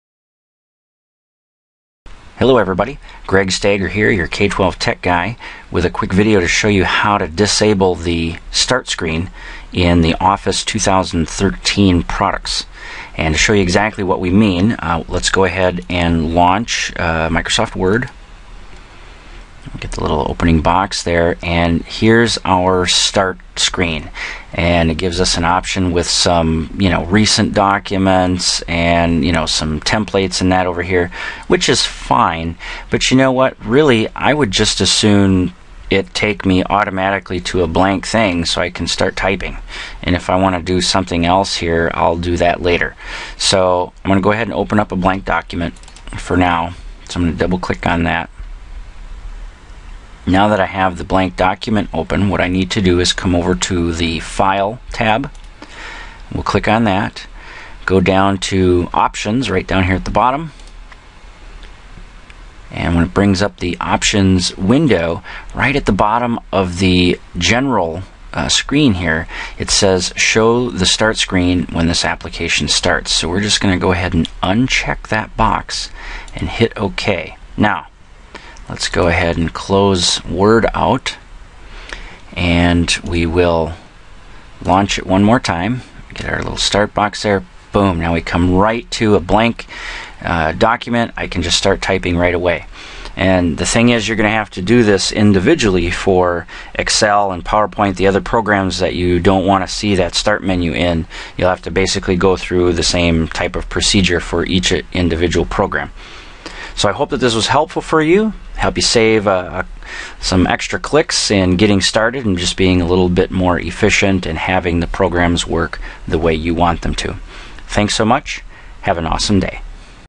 Disable Office 2013 Start Screen
Tutorial on how to disable the start screen so that just a blank workspace shows up after launch.